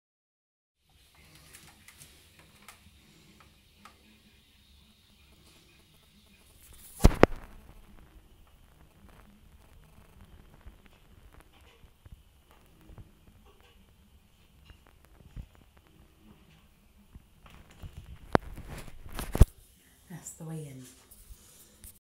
Weigh in
This is the weigh n last weigh in I didn't post but I was 338
I had gained 8lbs this weigh in means I lost 4lbs I'm happy